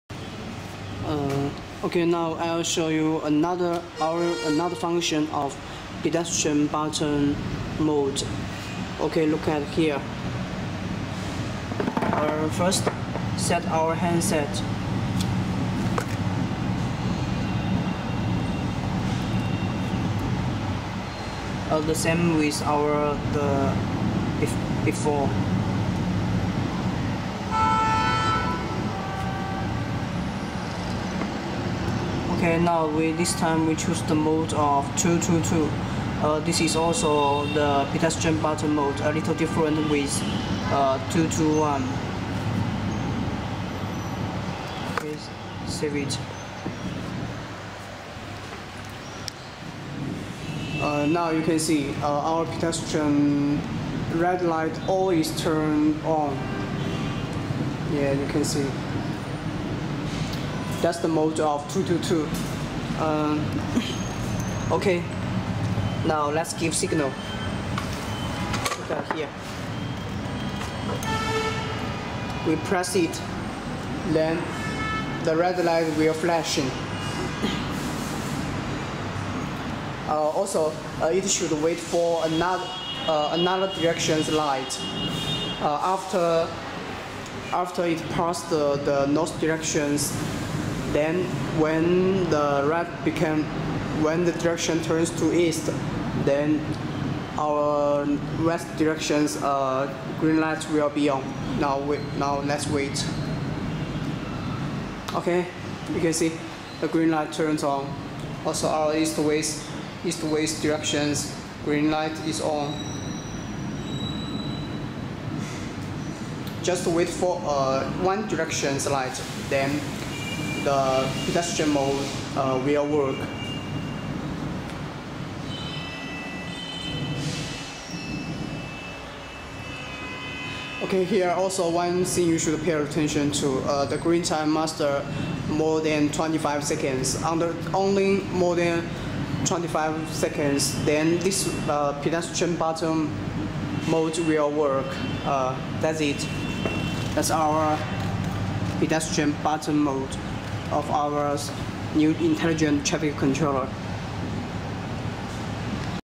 With Pedestrian push button function ---- Intelligent traffic control system
Mian function of Noble's Intelligent traffic control system:
Easy for maintenance and function extension by adopting modular design
Outdoor cabinet equipped with lighting protection and power filtering device.
Can control over 100 intersection by the Center office
Can work with Vehicle detector for more convenient management
Can work with pedestrian push button for the walking people
The green wave function is available
WIFI, GPRS, Ethernet, RJ45 function is available
RS-232, RS-485 to connect to computer for on-line adjustment, check and setup.